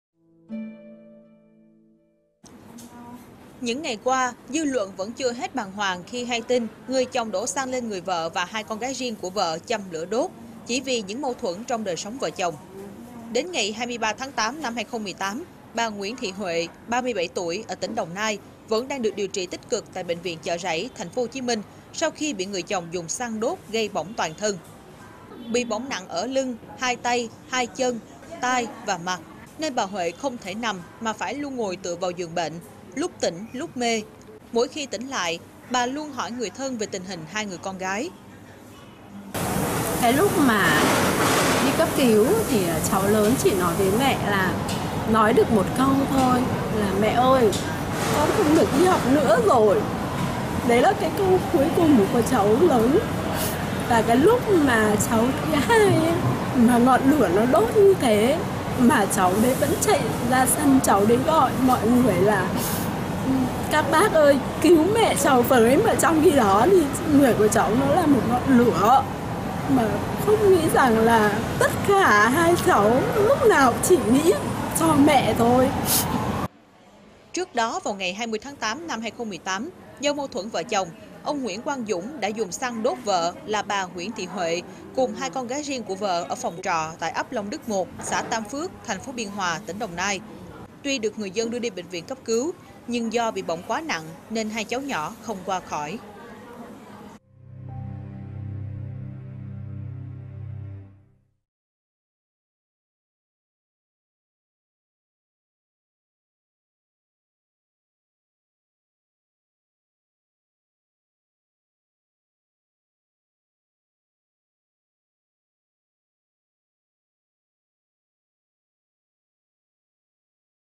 Bi kịch chồng dùng xăng thiêu sống vợ và 2 con riêng
Những ngày qua, dư luận vẫn chưa hết bàng hoàng khi hay tin người chồng đổ xăng lên người vợ và hai con gái riêng của vợ rồi châm lửa đốt, chỉ vì những mâu thuẫn trong đời sống vợ chồng.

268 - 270 Nguyễn Đình Chiểu, Phường 6, Q.3, TP HCM